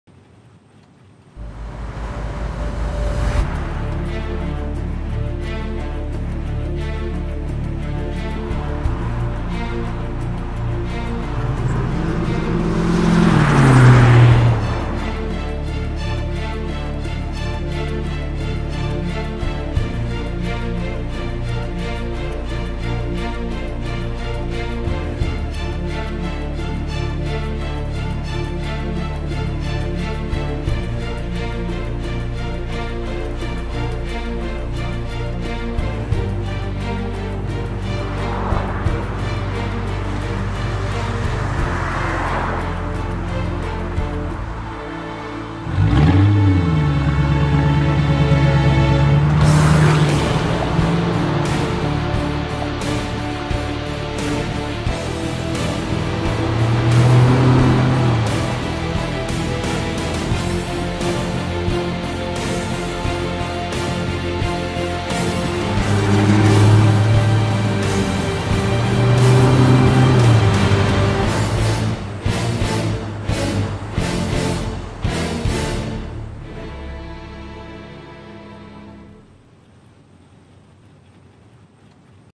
Rvinyl.com Presents: Aero Function - Luxury Body Kits
Aero Function lucxury body kits are made by Extreme Dimensions in the USA and are custom designed to fit like a glove. Made for the enthusiast and built to amaze, these kits are available in real carbon fiber, fiberglass and polyurethane.